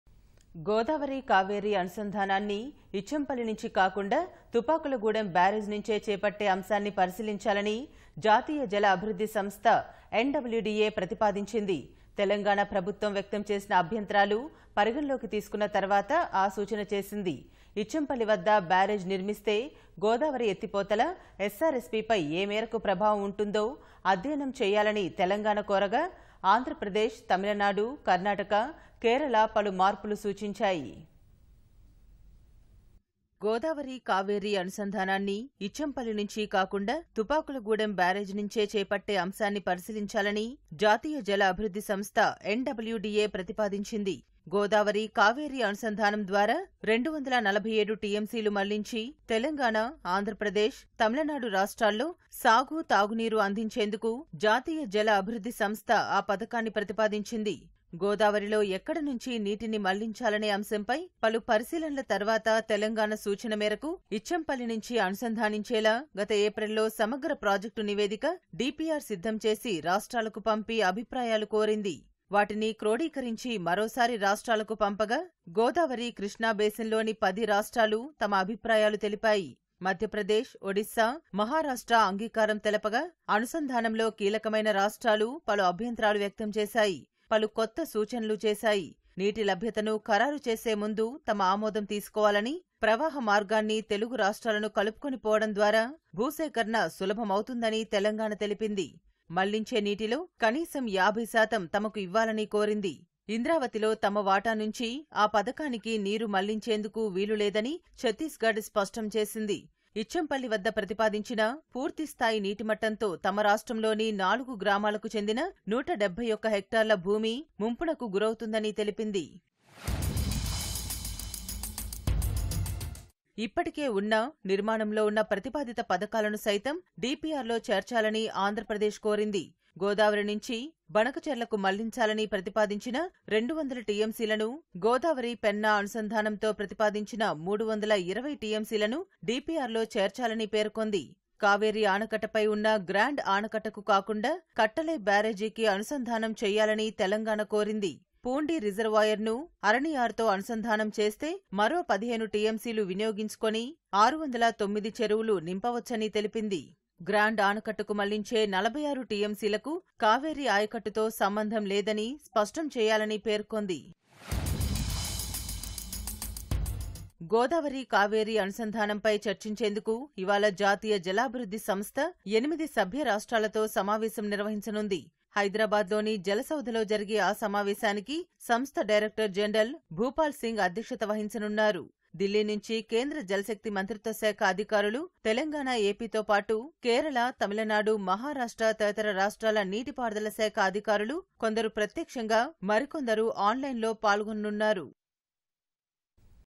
గోదావరి-కావేరి అనుసంధానంపై NWDA ప్రతిపాదనలు | NWDA Proposals on Godavari-Cauvery Connectivity
గోదావరి-కావేరి అనుసంధానాన్ని ఇచ్చంపల్లి నుంచి కాకుండా. తుపాకులగూడెం బ్యారేజీ నుంచే చేపట్టే అంశాన్ని పరిశీలించాలని. జాతీయ జల అభివృద్ధి సంస్థ-N.W.D.A. ప్రతిపాదించింది. తెలంగాణ ప్రభుత్వం వ్యక్తం చేసిన అభ్యంతరాలు పరిగణనలోకి తీసుకున్న తర్వాత ఆ సూచన చేసింది. ఇచ్చంపల్లి వద్ద బ్యారేజి నిర్మిస్తే. గోదావరి ఎత్తిపోతల, SRSPపై ఏమేరకు ప్రభావం ఉంటుందో.. అధ్యయనం చేయాలని తెలంగాణ కోరగా. ఆంధ్రప్రదేశ్, తమిళనాడు, కర్ణాటక, కేరళ పలు మార్పులు సూచించాయి..